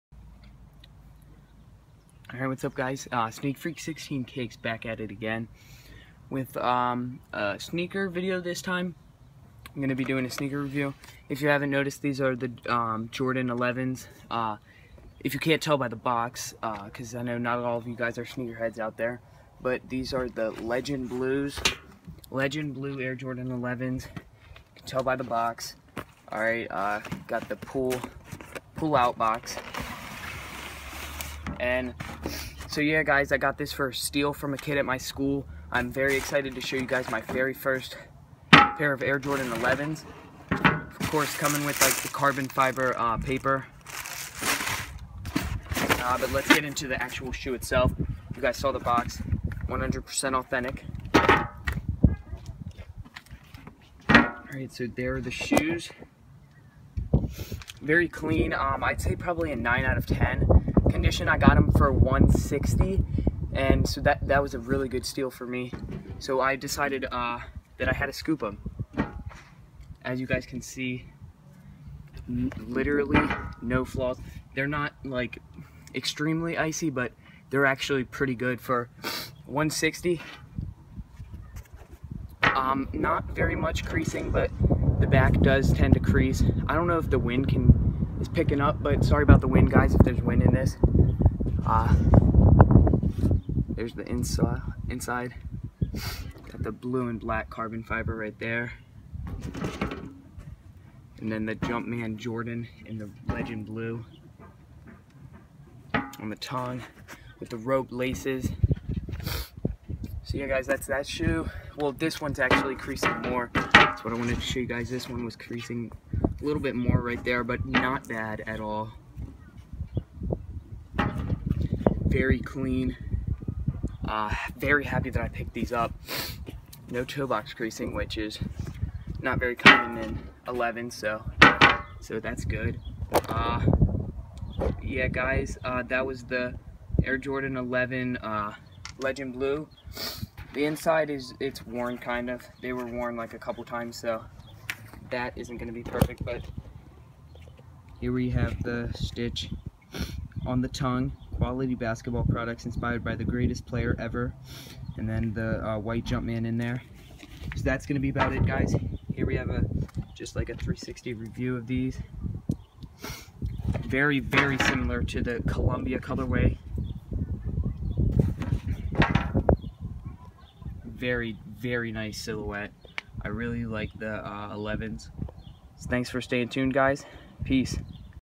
Air Jordan 11 legend blue
Got a great do on these for 160$!!! Tell me what y'all think??
S/o to sblake
I know y'all saw Kris Jenkins with the 3 point buzzer beater last night what an upset!